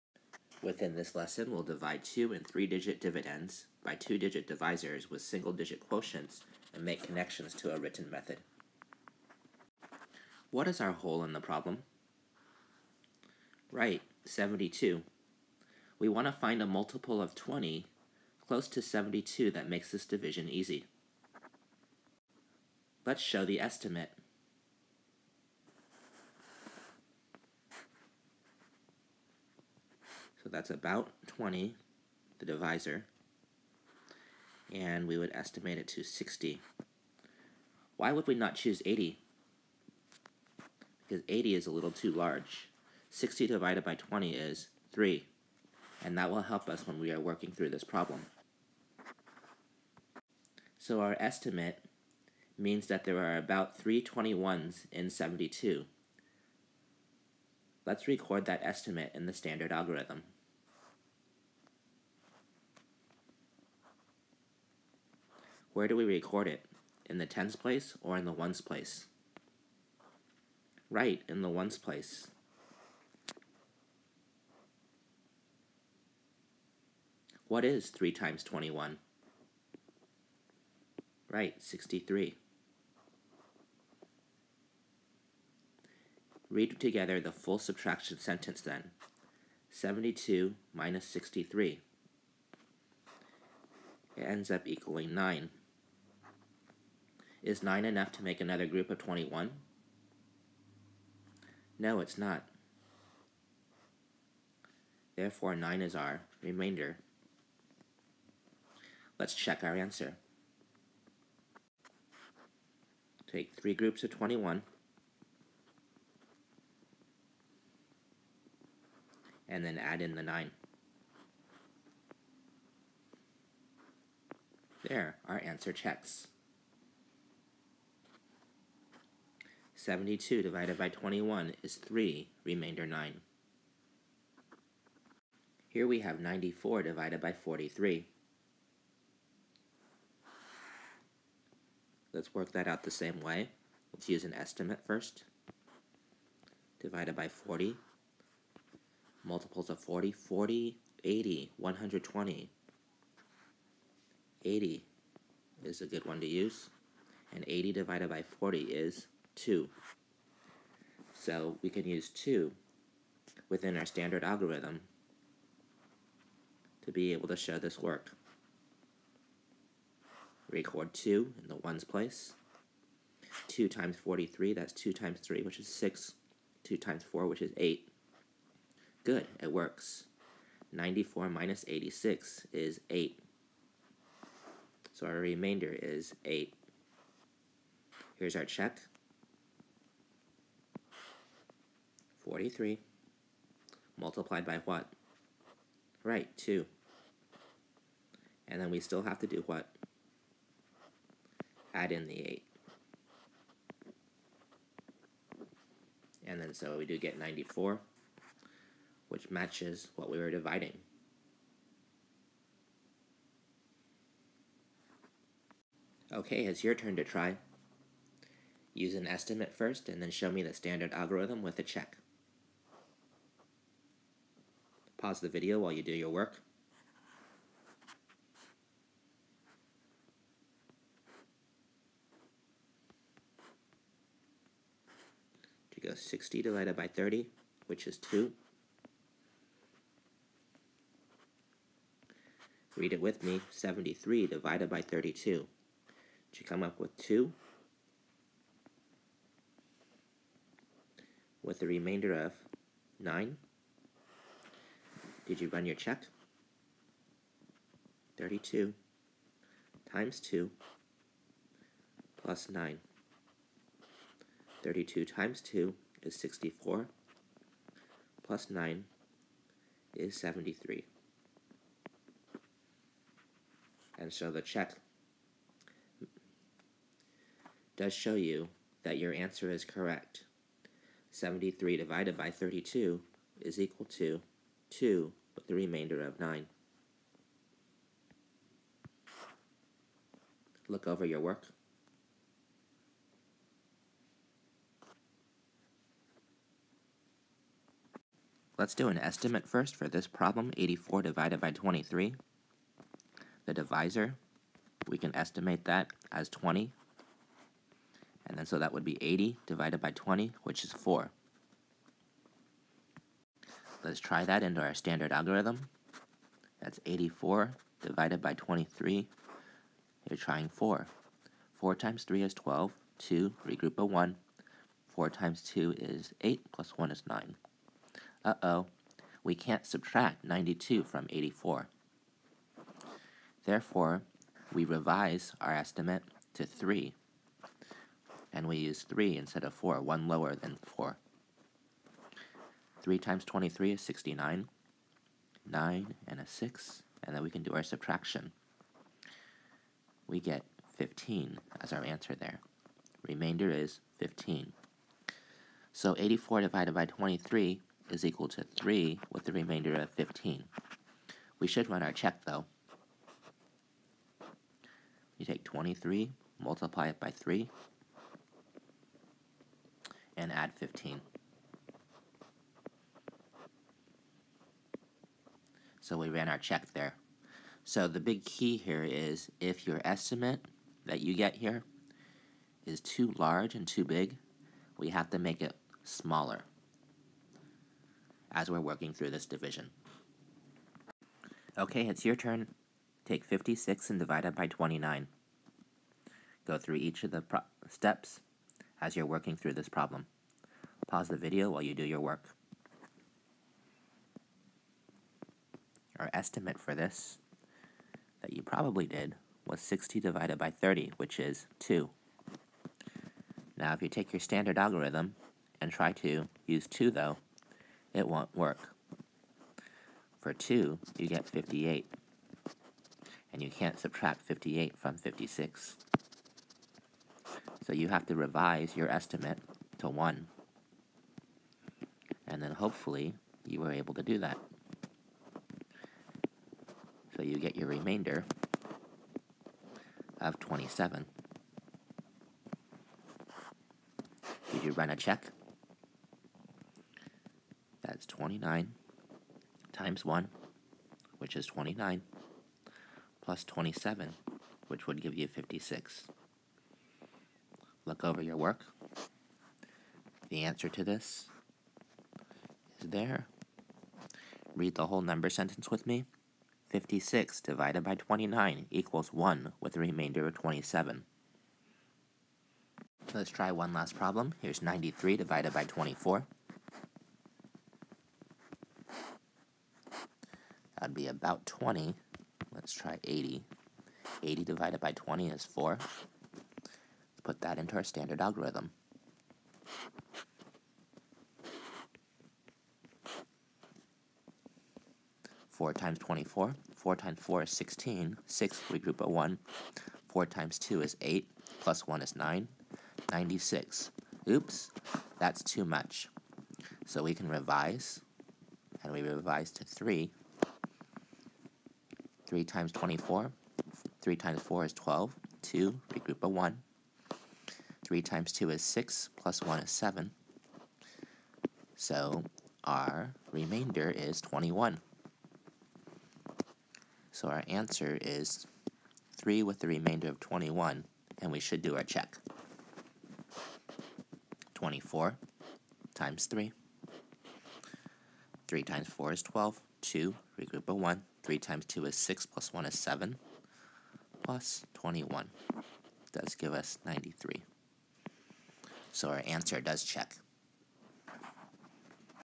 Engage Ny Module 2 Lesson 20
Divide by two digit divisors with connection to a written method